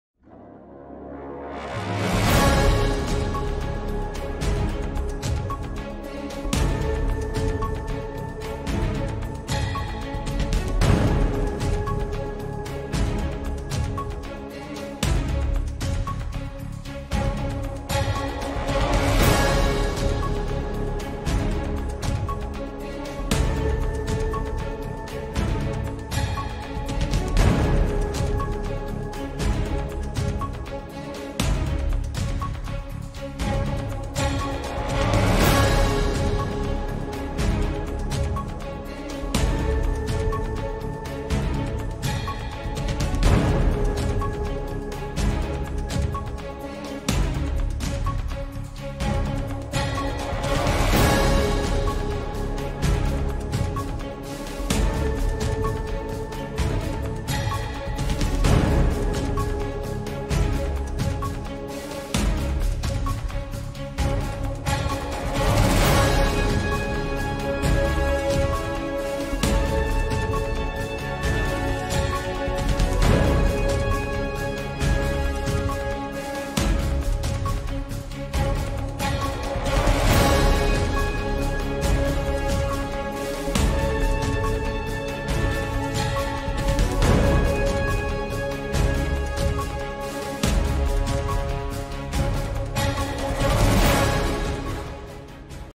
Estonian Police Chase in Viivikonna | 21.08.2022
The moped driver ran away from the police because he mistakenly thought he was drunk